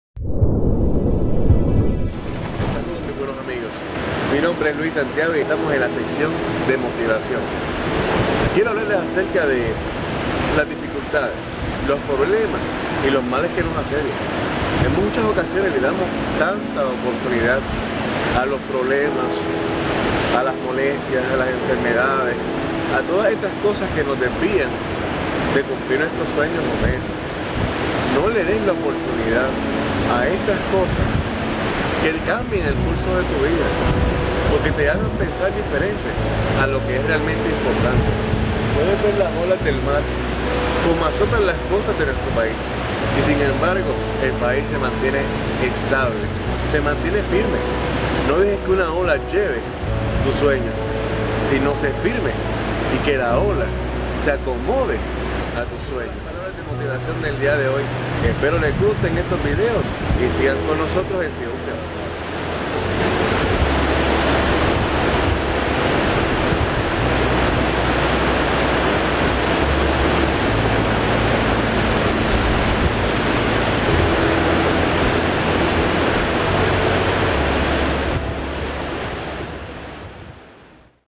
Motivación (Sé firme)
Saludos mis amigos,desde las hermosas playas de Rincón Puerto Rico. Les dare palabras que inspiraran firmeza en tiempos de tempestad. La mente se nubla cuando hay problemas, tristeza y soledad. En los momentos más difíciles recuerda quien eres y para donde vas.

Para participar por la GoPro hero 6

3. Comenta